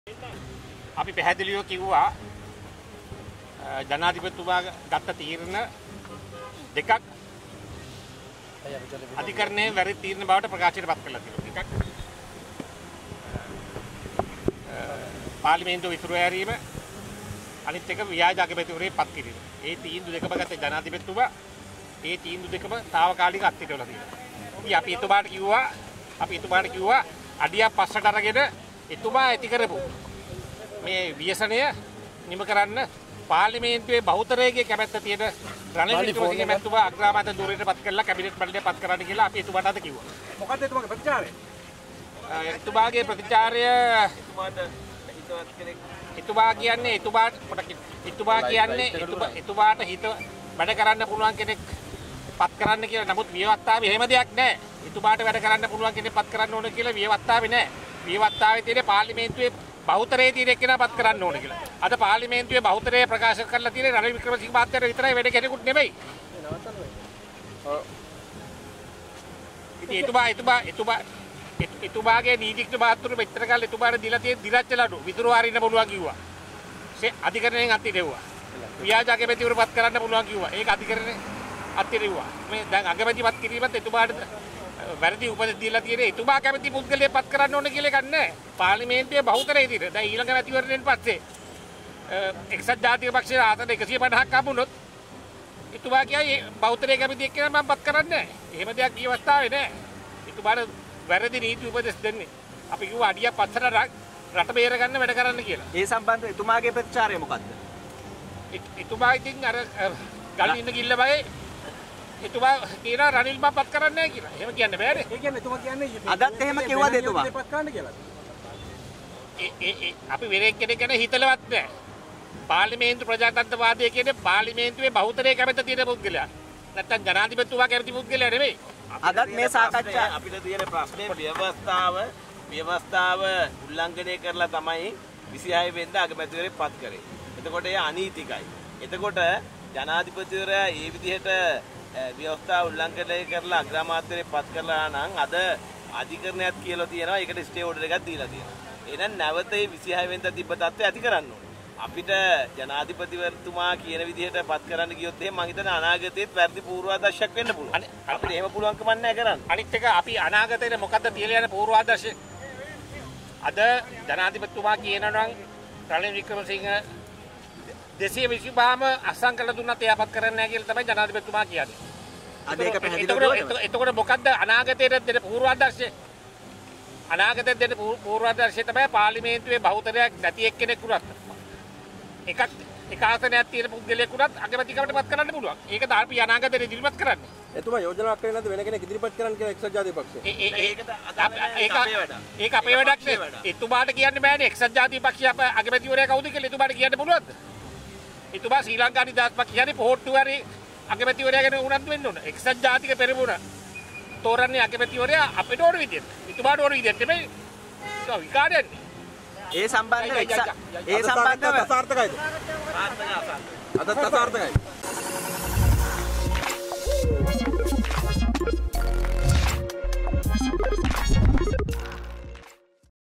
එ.ජා.ප අගමැති තෝරන්නේ අපිට ඕනි විදියට එතුමාට ඕනි විදියට නෙවේ මේක මහා විකාරයක්නේ | Breaking.lk
එ.ජා.ප අගමැති තෝරන්නේ අපිට ඕනි විදියට
එතුමාට ඕනි විදියට නෙවේ මේක මහා විකාරයක්නේ